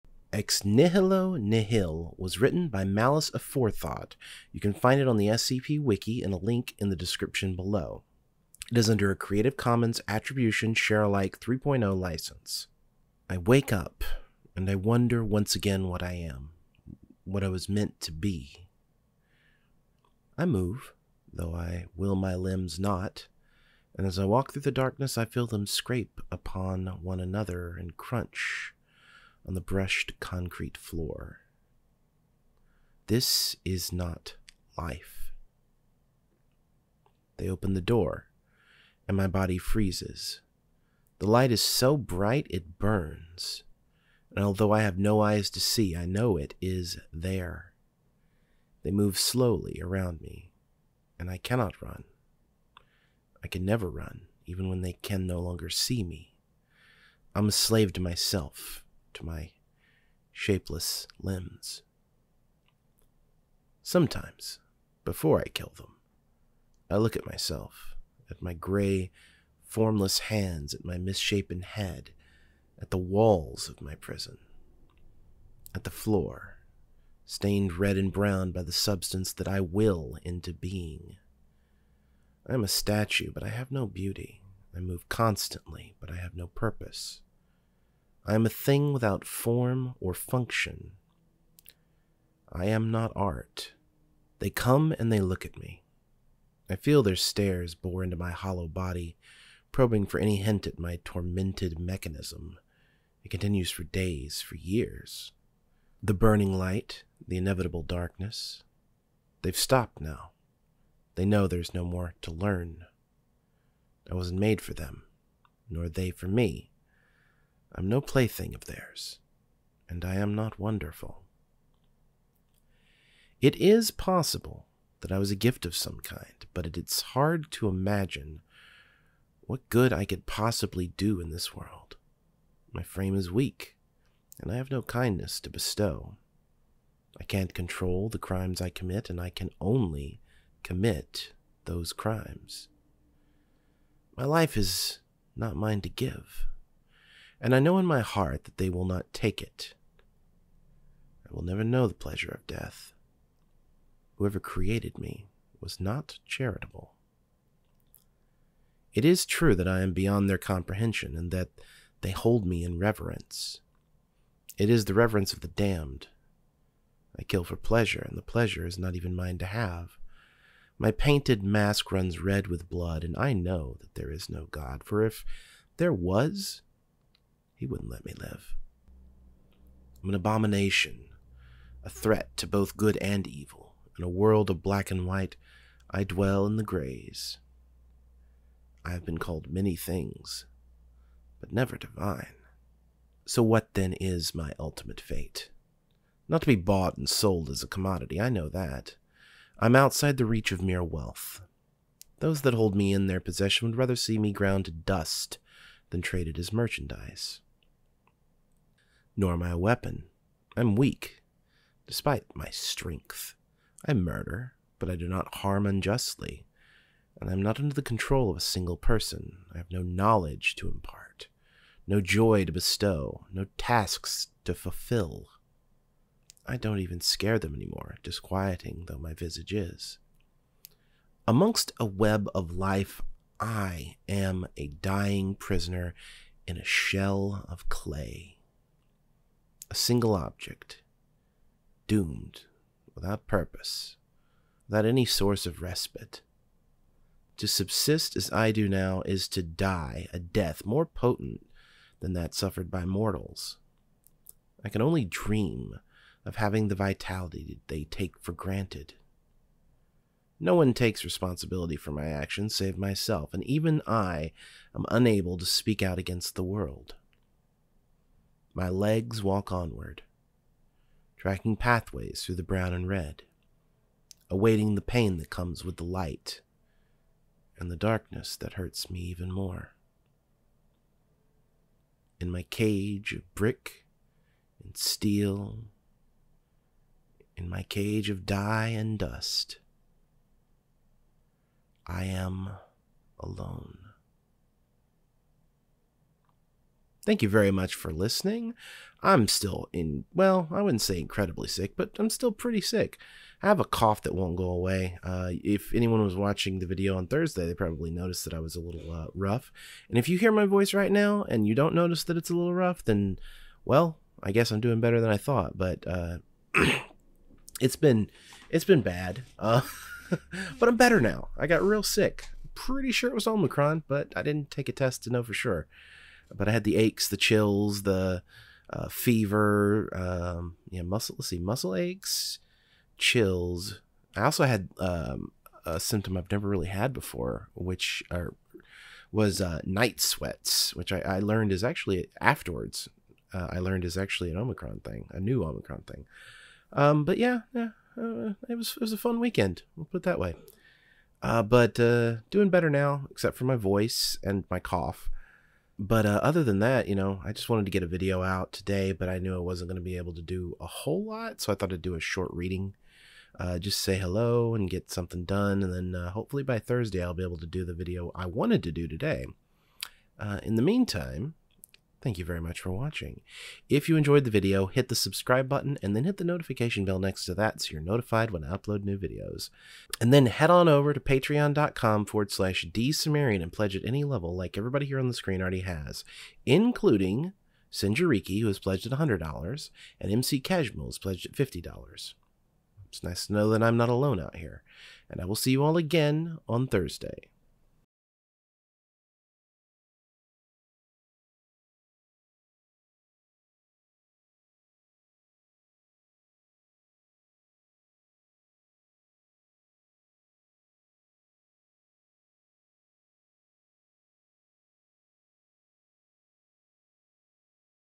SCP-173 is Having a Crisis
SCP-173 needs some help with a crisis he's having.

The tale featured in this video is Ex Nihilo Nihil by MaliceAforethought, and can be found

Thumbnail image of Doctor Cimmerian was commissioned from Lord Bung.